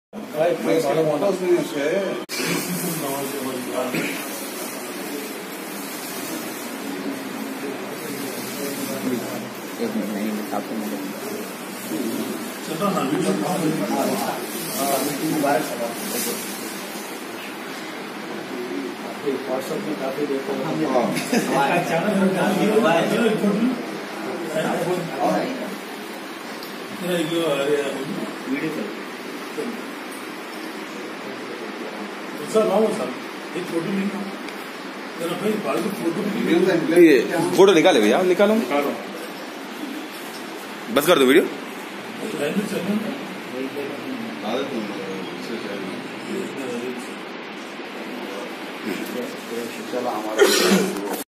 Waheed Ahmed Advocate Member TSWB felicitated to Our Beloved Party Leader Ex Minister Janab Mohd Fareed Uddin Sahb Newly Nominated as a Post of Chairman (TSAHCMW ) Telangana State Assembly House Committee Minorities Welfare Departments